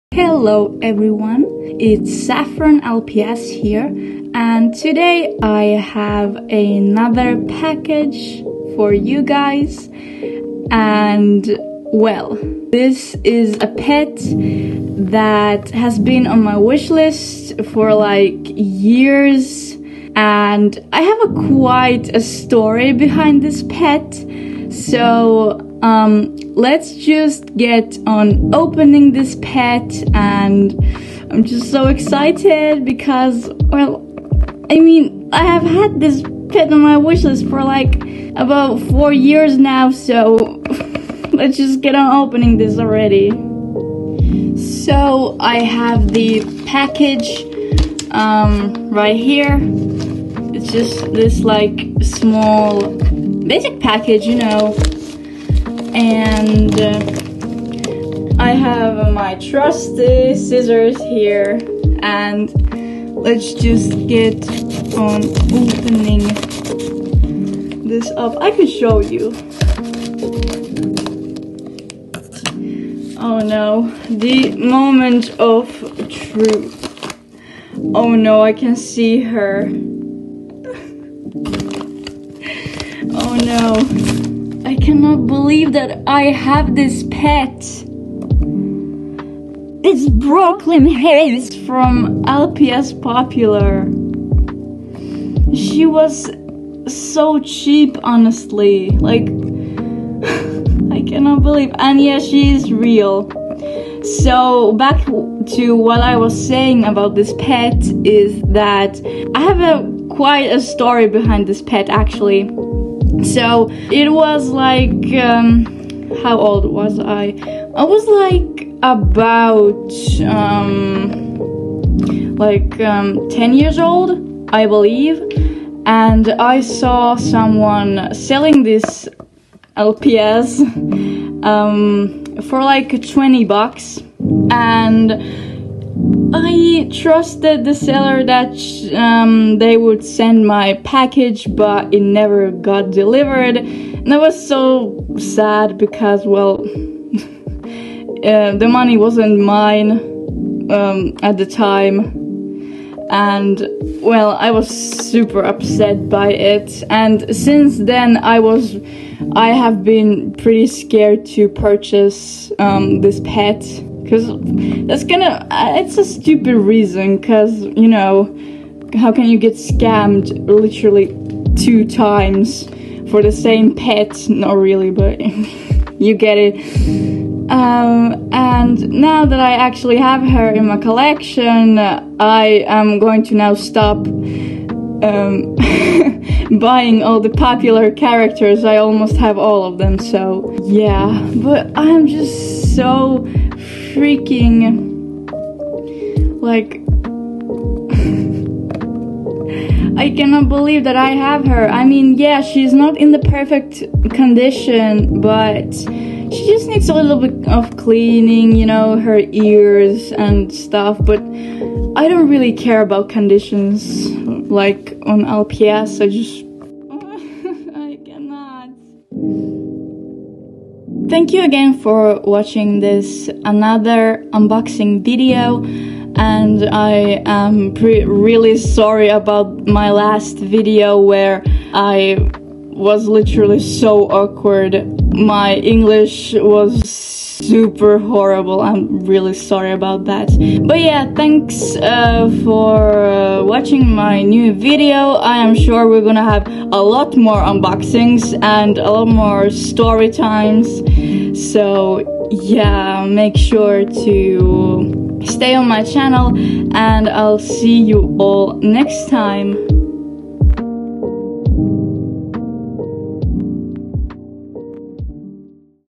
Lps package opening/Unboxing my dream rare lps!
Hello everyone!
So this time I have a story time behind this lps. It unfortunately did not end well :/. But now things are looking brighter again regarding to this lps.